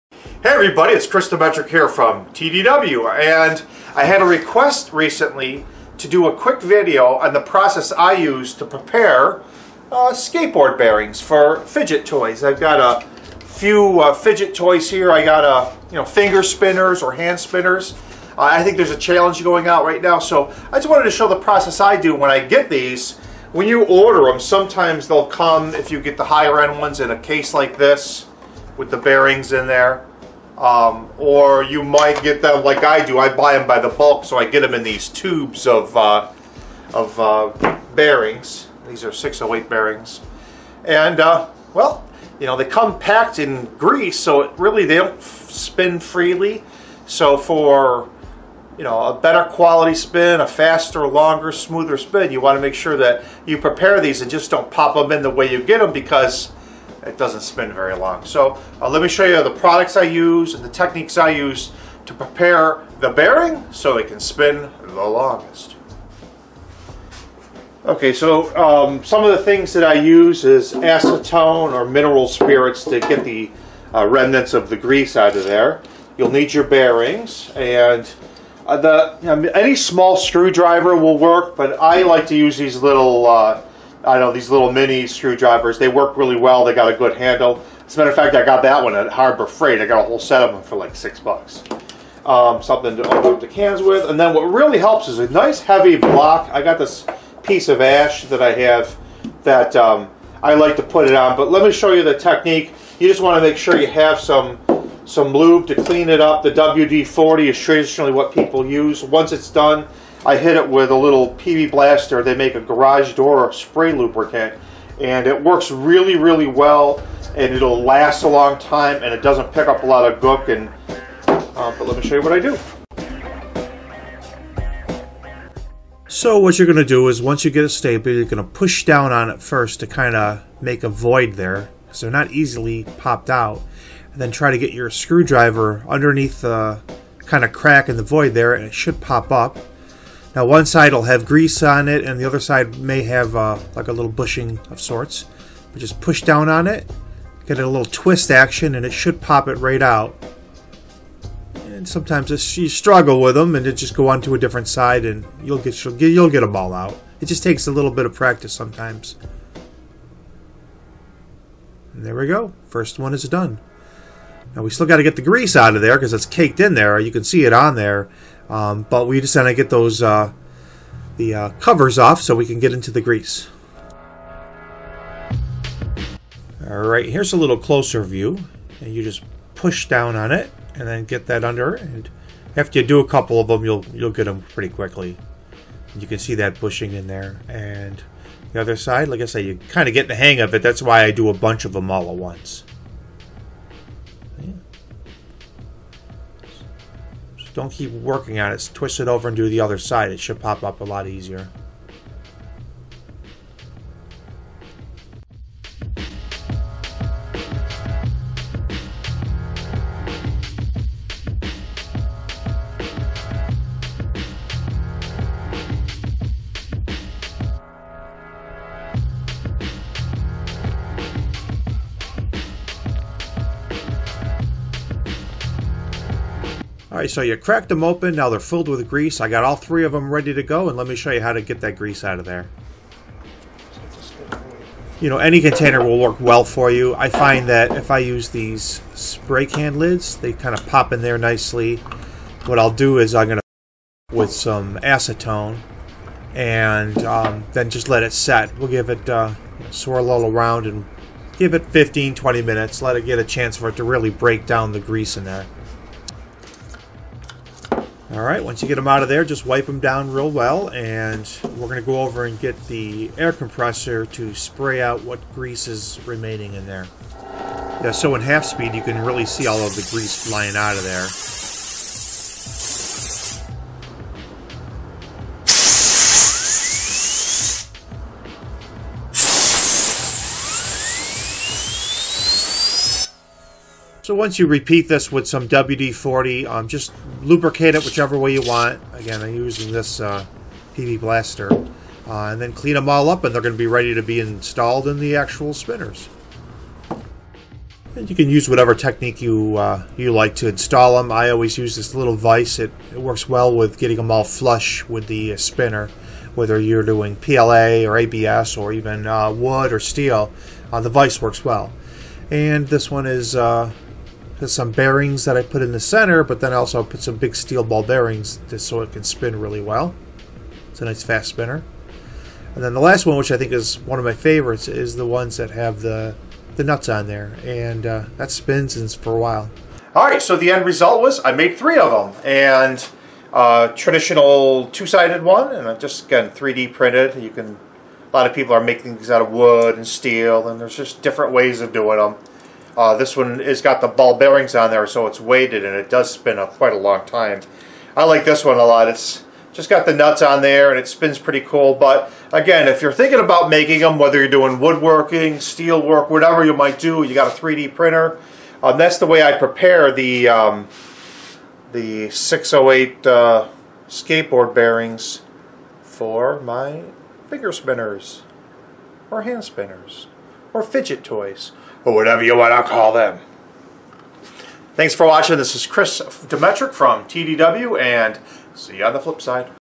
How I prepare my bearings for fidget spinners
Per request a video on getting 608 bearings ready for a spinner
TDW Woodworks